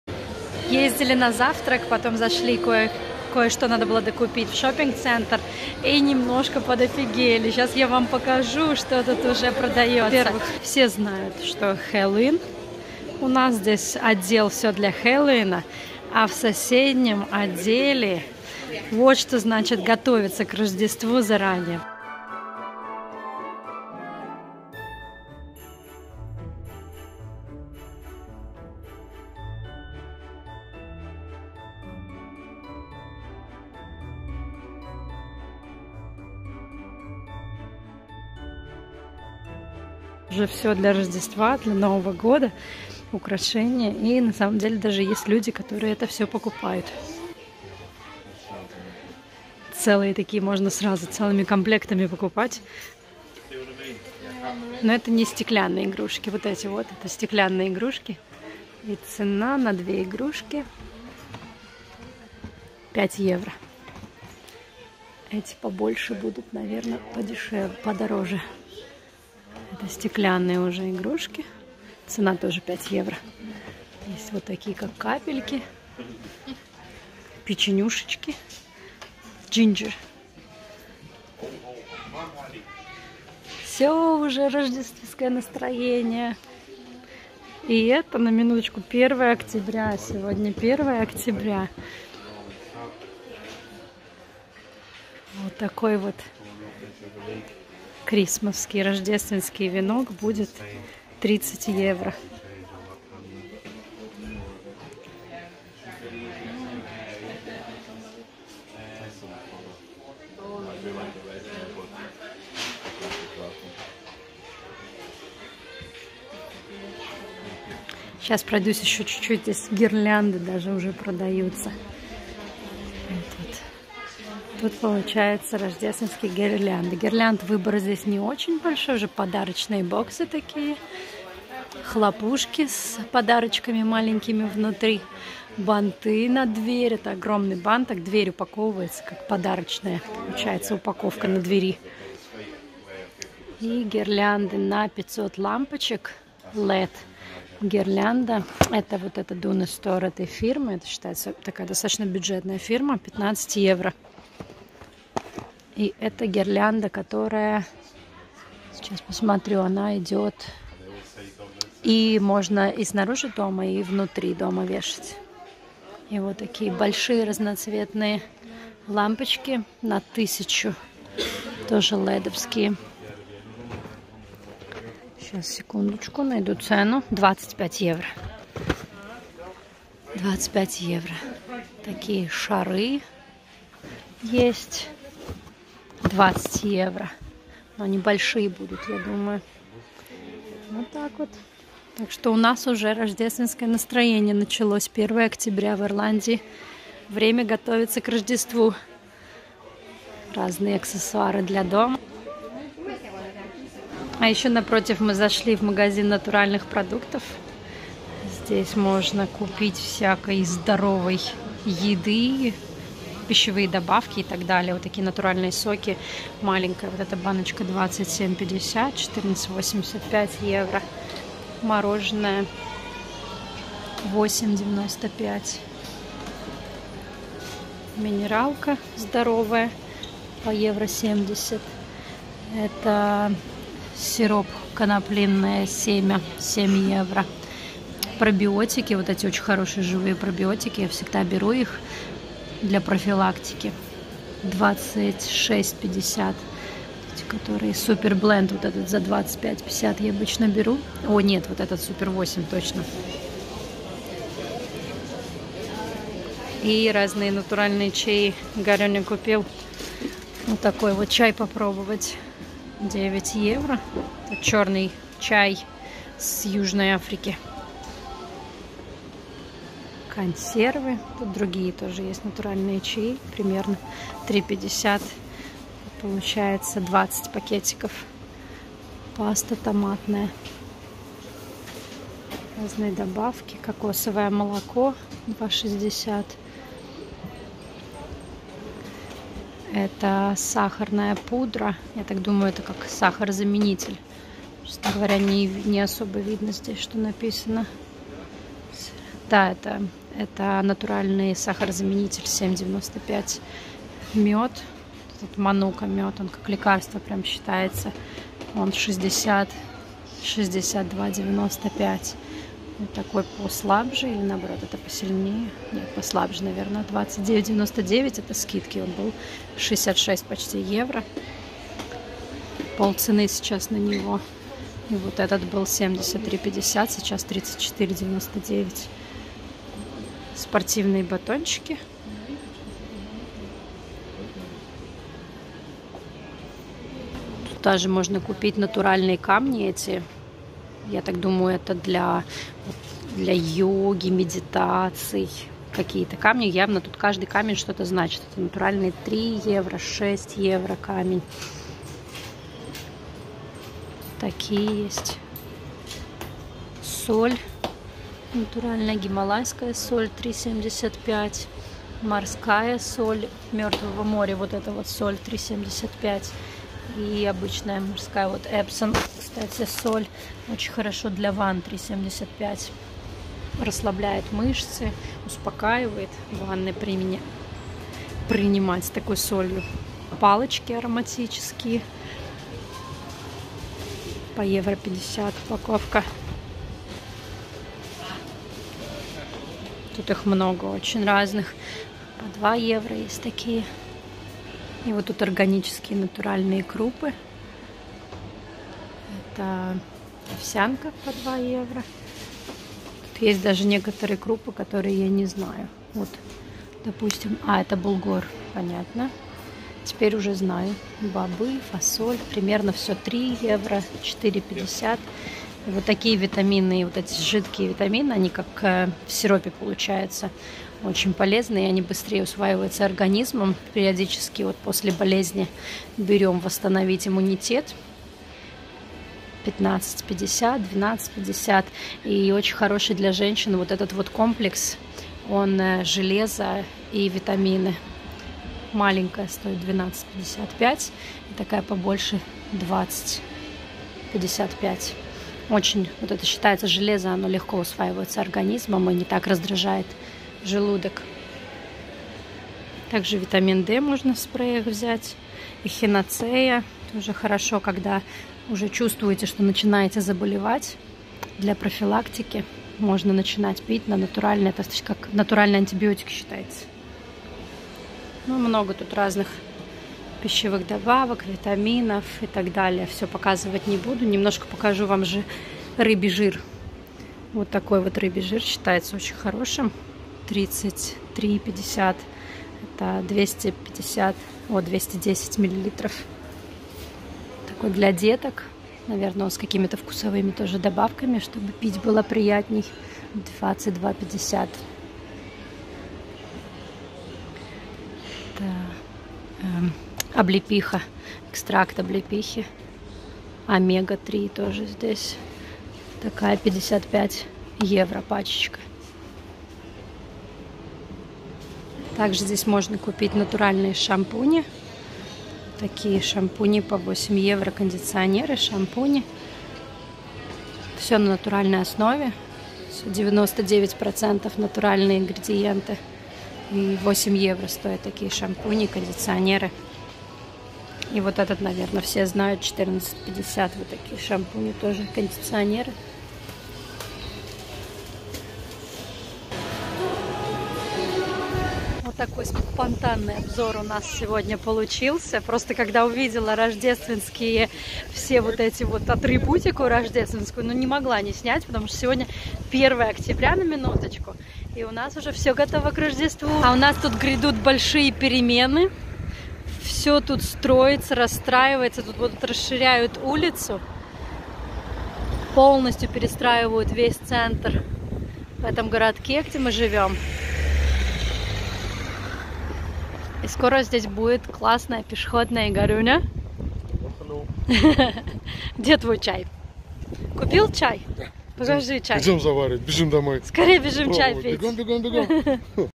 🇮🇪 Готовим сани летом или любимый Ирландский праздник 😃 Спонтанный выпуск.
Вот это да. Ирландия продолжает удивлять! 😁